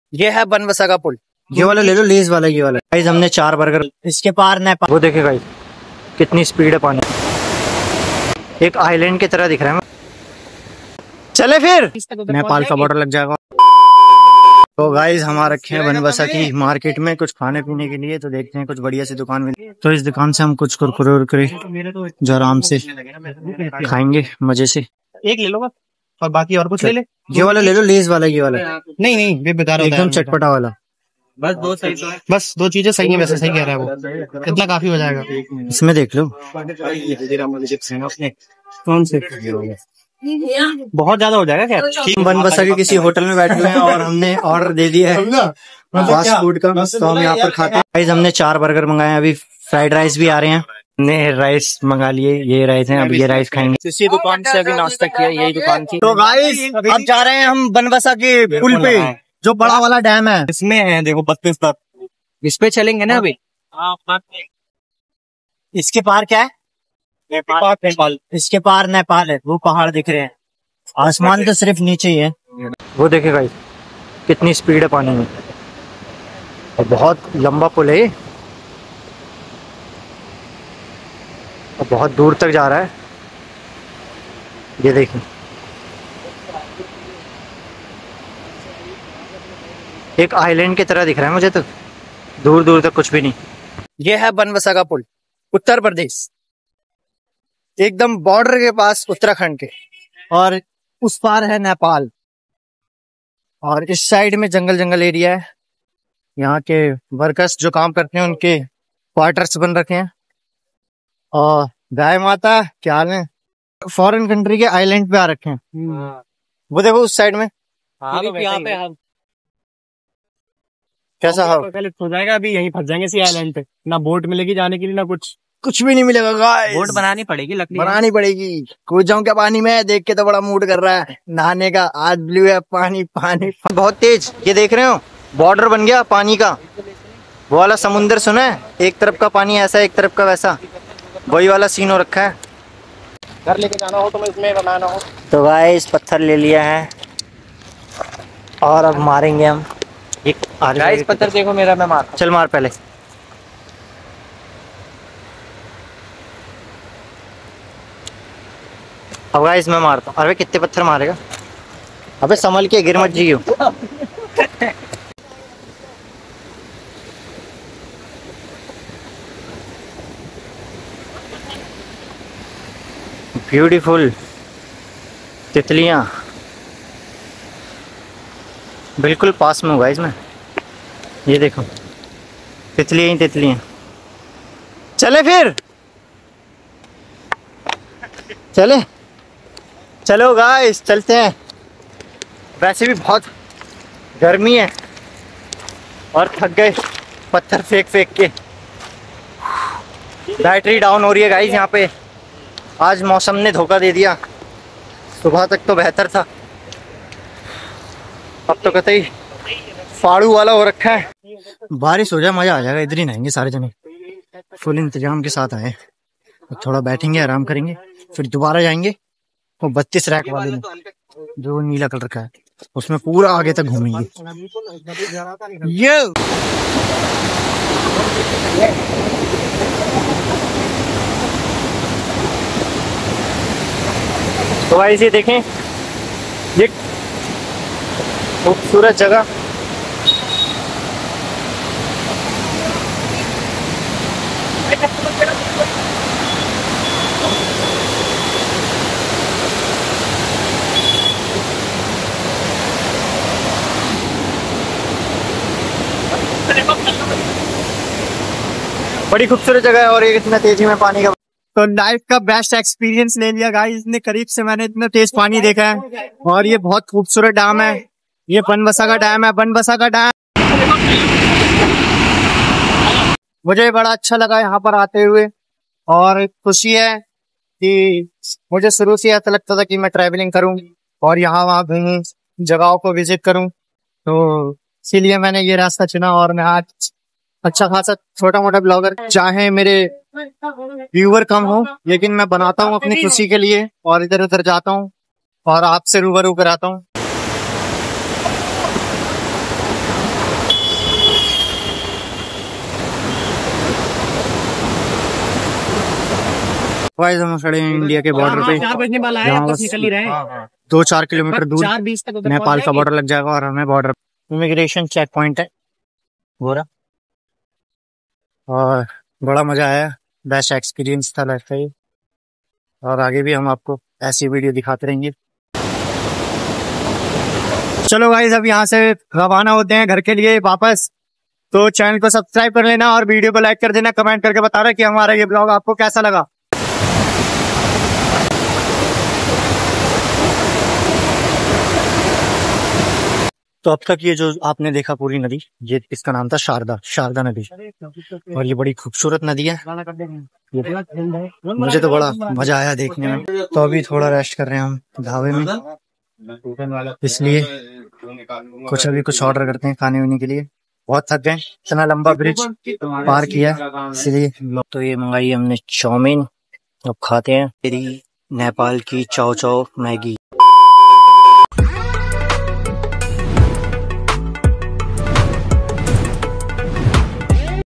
BANBASA BARRAGE DAM (SHARDA RIVER)
HELLO 👋
GUY'S
IM ALTAMAS AHAMD VLOGGER

GUYS AAJ MAI AAYA HU INDO NEPAL KE BORDER PAR JO BANBASA KE PASS SHARDA RIVER PAR JO HIMALAYA SE NIKAL KAR UTTARAKHAND OR UTTARPRADES MAI BEHNE WALI RIVER HAIN OR YE BAHUT ACHHI HAI AAP ISS VLOG PURA DEKHNA HAI OR BATANA HAI KI KAISA LAGA AAPKO YE VLOG